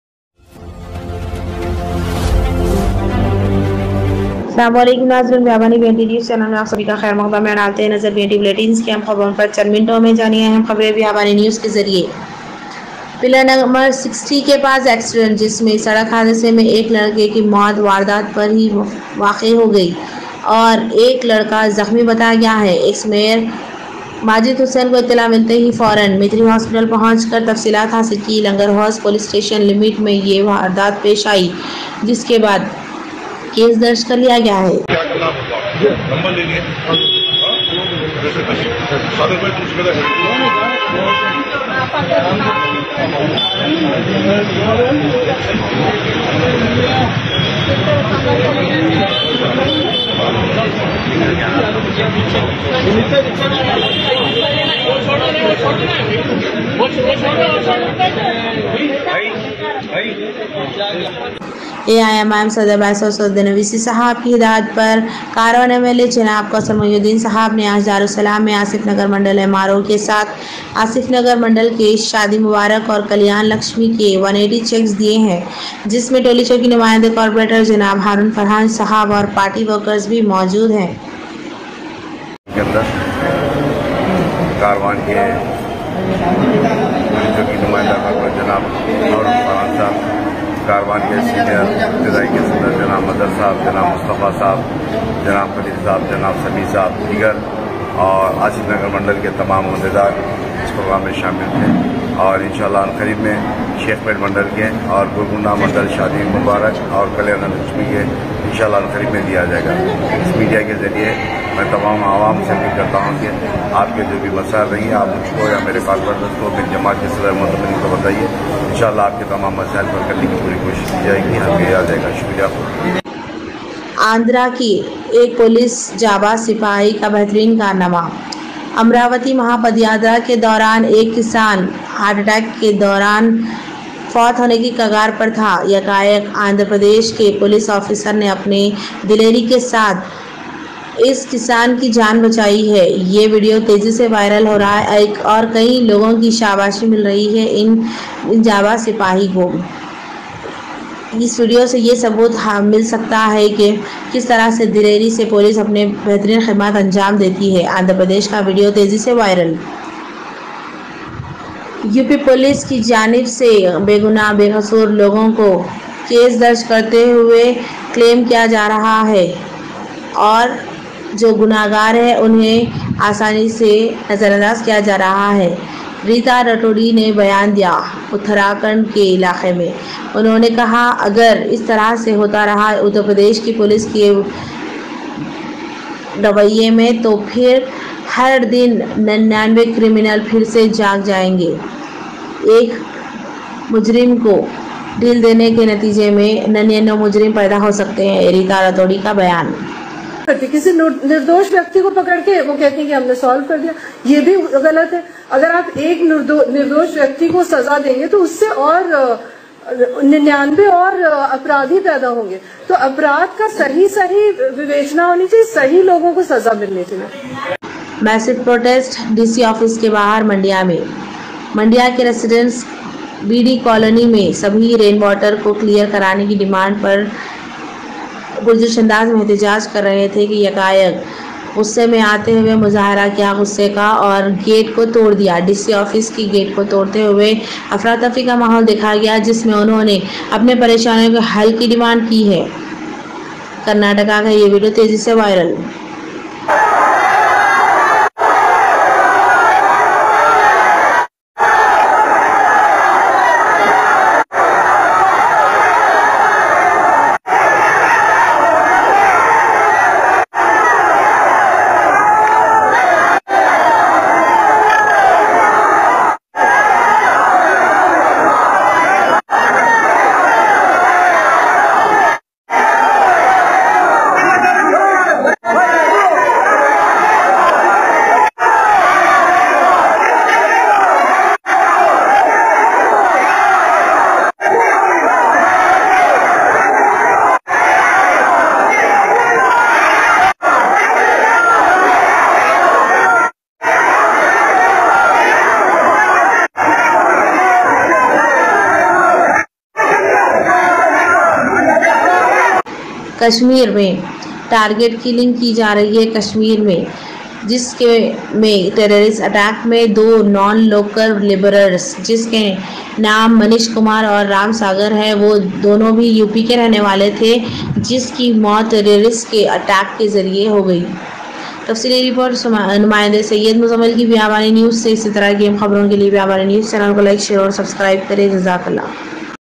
Sadakhad*sa me ek Naujawan maukhe per Faut A.P cop ne Jaan
BNTBulletins LangerHouse Sadakhad*sa me ek Naujawan maukhe per Faut A.P cop ne Jaan bachayi kissan ki

PILLAR NO 60 KE PASS ACCIDENT ME EK LADKA KE MOQUE WARDAAT PAR HE MOUT WAQE HOGAYE OR EK LADKA ZAKHMI BATAYA GAYA HAI EX MAYOR MAJID HUSSAIN KO ITELA MILTE HE FAURAN MYTHRI HOSPITAL PONCH KAR TAFSILAAT HASIL KI LANGAR HOUZ POLICE STATION LIMITS

AIMIM President aur MP Hyderabad Janab Br Asaduddin Owaisi ki hidayath par AIMIM karwan Mla Janab Kausar Mohiuddin ne Aaj Darussalam mai Asif nagar mandal MRO k saat Asif Nagar mandal k shadi Mubarak aur Kalyana Lakshmi k 180 cheques diye hai jis mai Tolichowki Numainda Corporator Janab Haroon Farhan sahab aur party workers maujud hai

Andhra cop pumps life back into a farmer part of AmaravatiMahaPadayatra, wins hearts as the video goes viral. A Farmer suffered a heart attack during the walkathon, witnessing this a police inspector performed CPR which saved his life.

At times UP police apprehends an innocent person and claims the case is solved. If you punish one innocent man, 99 criminals will be born: Rita Ratudi, ACS Home Uttarakhand

Massive protest happened at DC office in Mandya by residents of BD colony demanding rain water to be cleared from their areas.They broke the main gate and tried to barge into DC office demanding solution.Incessant rains have forced some of them to move 4m their homes

Targeted killings in Kashmir continue, two non-local labourers namely Manish Kumar & Ram Sagar both from UP were killed as terrorists lobbed grenade in Shopian.

Shopian, J8,,K 12 people arrested.They've confessed to committing crime.Main accused,a LeT terrorist,for whom arrested accused works,to be neutralised soon.Non-local labourers are soft targets;they don't have security: ADGP Kashmir, on 2 non-local labourers shot dead by terrorists..

Bureau Report from Reporter Syed MUZAMMIL.